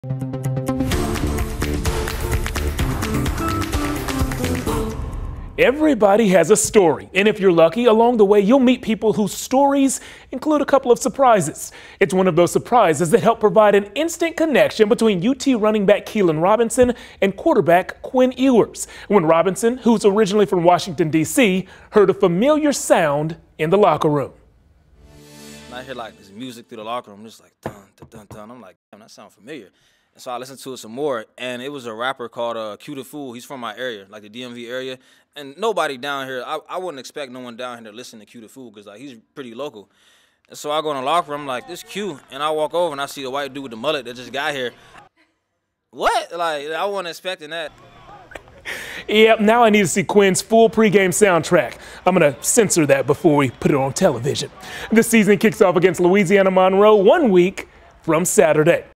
UT football players bond over East Coast rap | KVUE
Listen to UT running back Keilan Robinson share his story about Quinn Ewers.